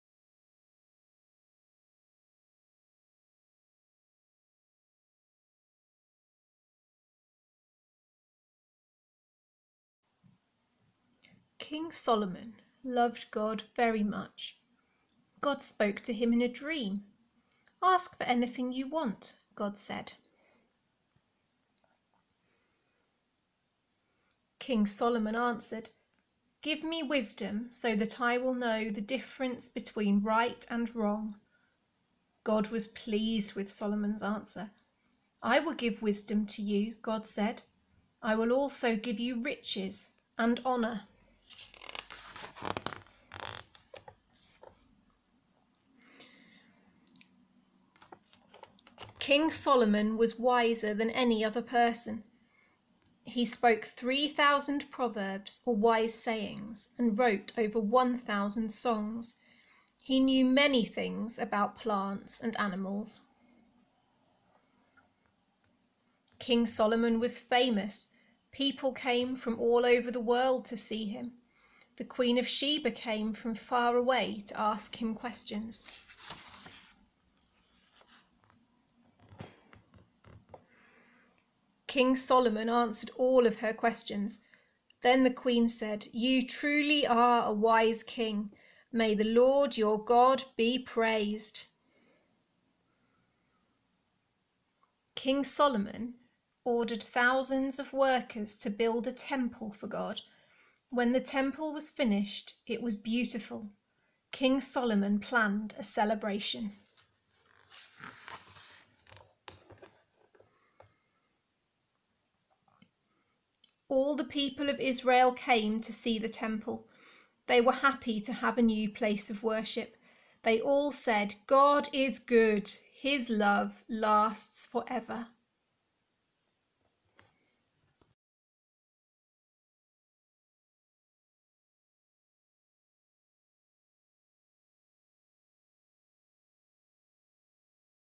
Jesse Tree No. 14 Solomon
Did you know that the Christmas story is part of a bigger story?

If you would like to read this story about about Solomon in the NIV translation in the Bible, read 1 Kings 1:28-37 and 1 Kings 3:1-15

Can you guess what item might have been made for today's story and hung up on the giant Jesse Tree outside St Peter's? Can you spot the item on the tree when you walk past? If you're making your own Jesse Tree at home, will you make the same item or choose something else?